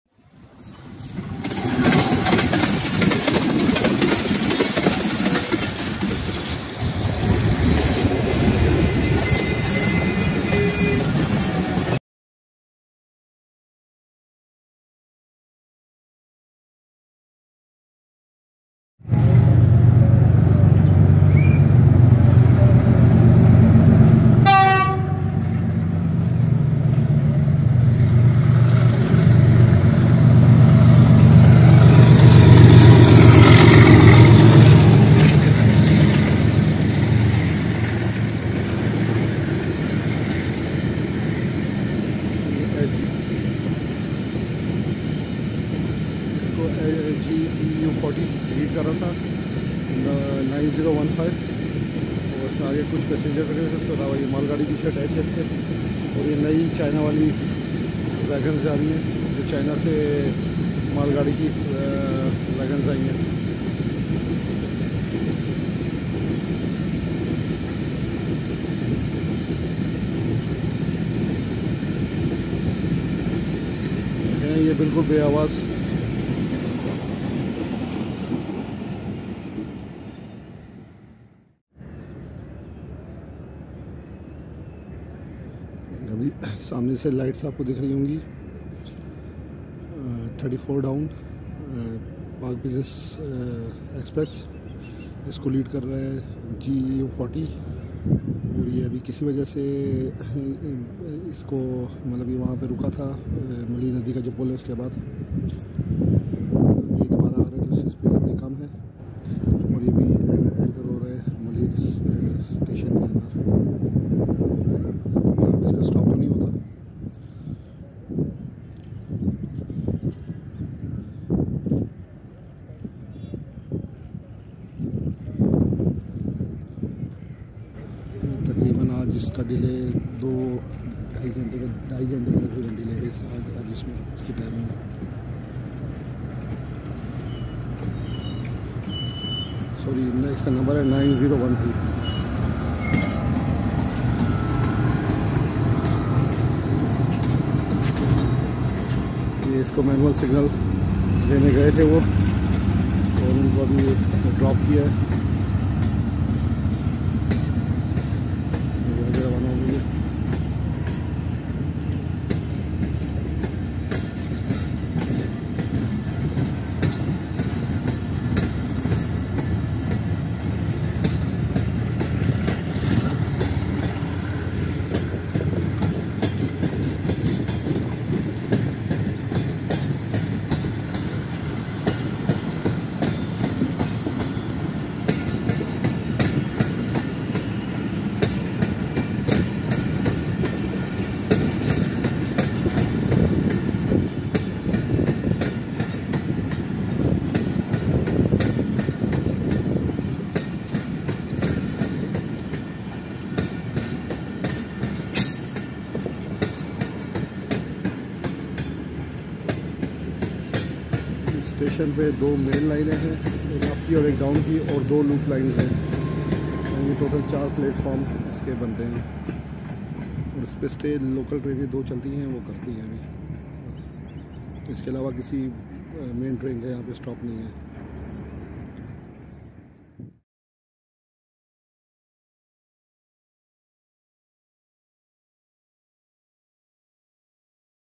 Newly Arrived Chinese Cargo Wagon & 34DN Pak Business Exp Passed Malir
Newly Arrived Chinese Cargo Wagon Lead by GEU 40 9015 & 34 DN Pak Business Express Passes Through Malir Station